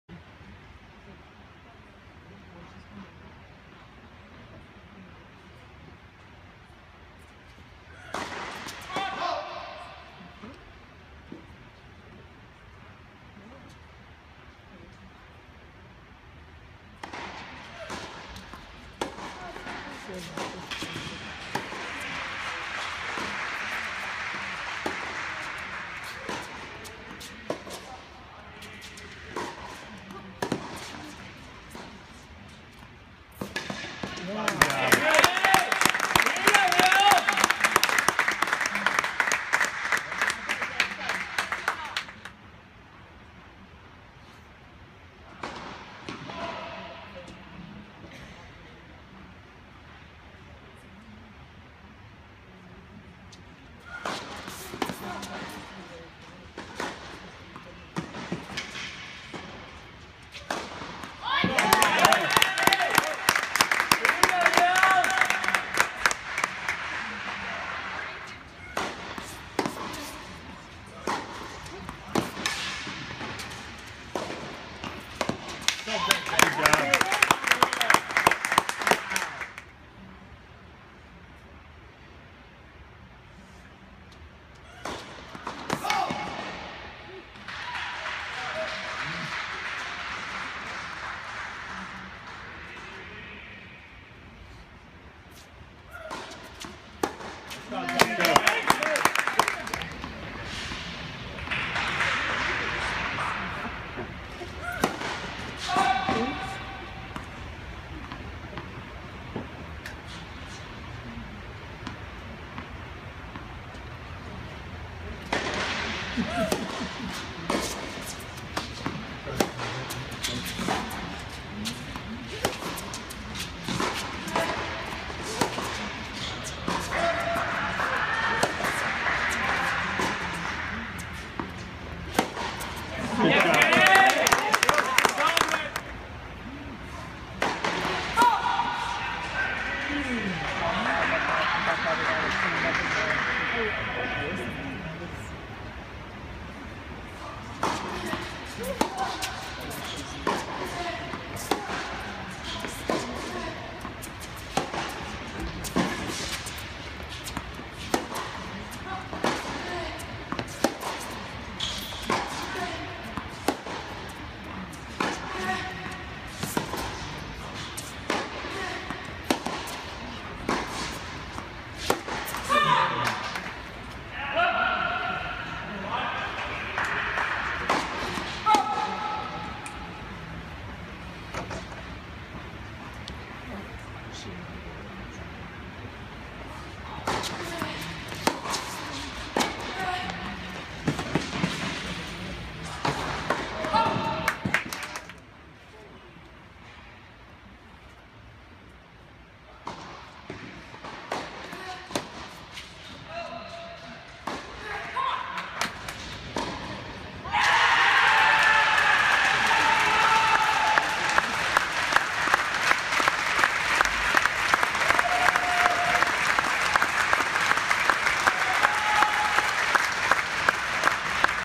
2015 NCAA Women's Singles Final
Jamie Loeb of North Carolina defeats Carol Zhao of Stanford 6-2, 4-6, 6-1, Baylor University, Waco Texas, May 25, 2015